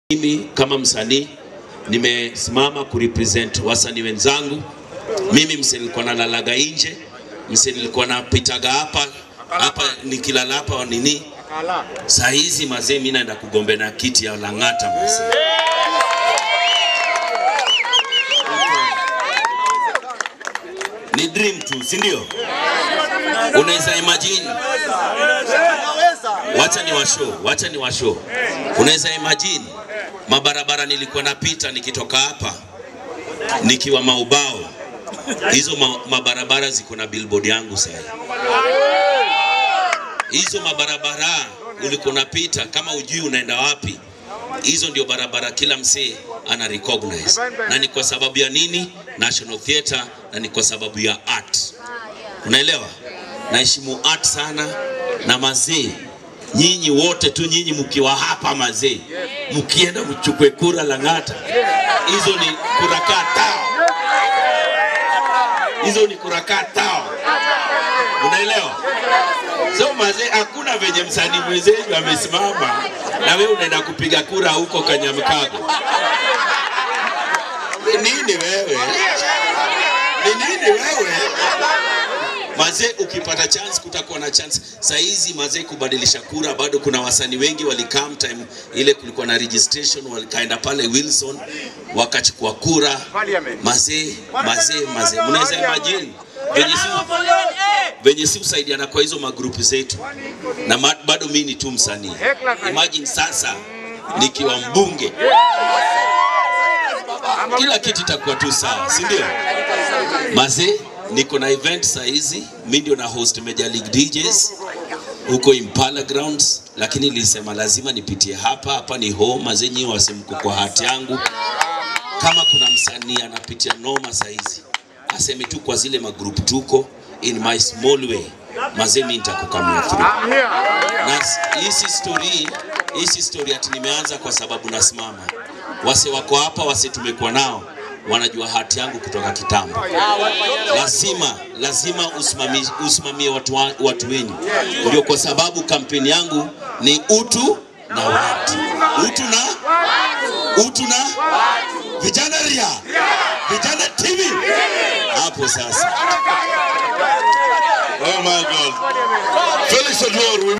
How Jalang'o became so famous through Kenya National Theatre.
From Comedy to now becoming a member of parliament is something that is not easy but Jalang'o has seen it all including sleeping the whole night outside the Kenya National Theatre doors with mwalimu Churchill. This shows humility and huble beginning to greatness.